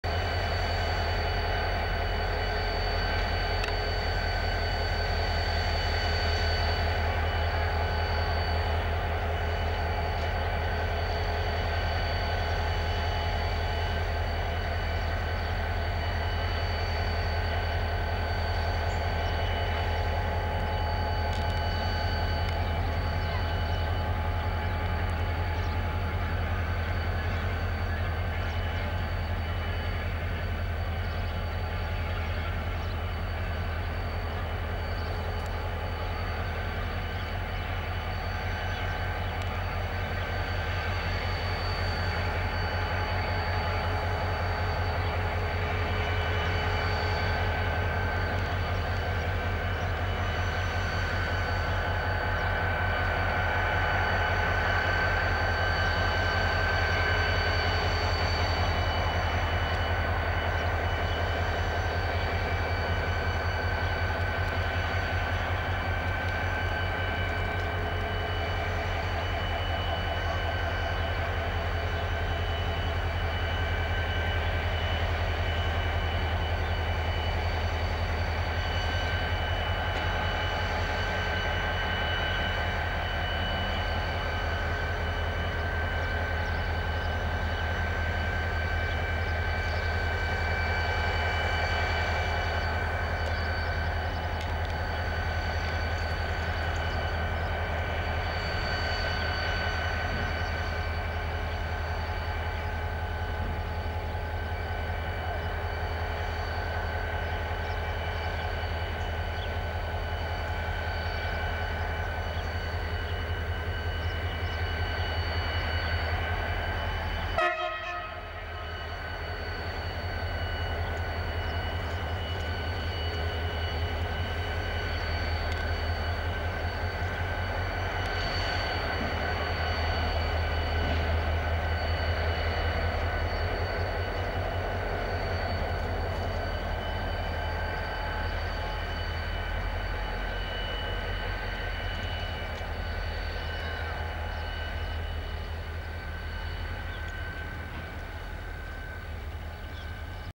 Renfe 333-082 Mdt Salamanca 1991
3382 gets rotated on the plate on Deposito sa 4.may 1991